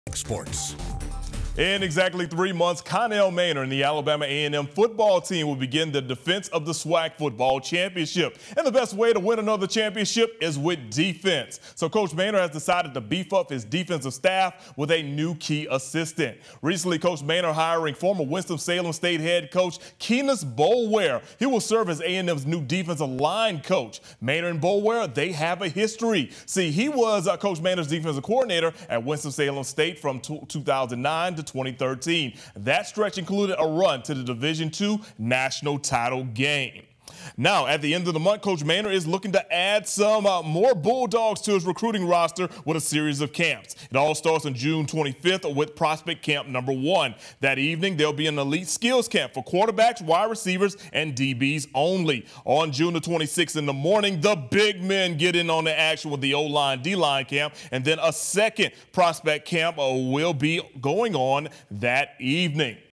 Alabama A&M hires Kienus Boulware to coach the defensive line
Kienus Boulware, a former head football coach at Winston-Salem State, has been hired as the defensive line coach at Alabama A&M.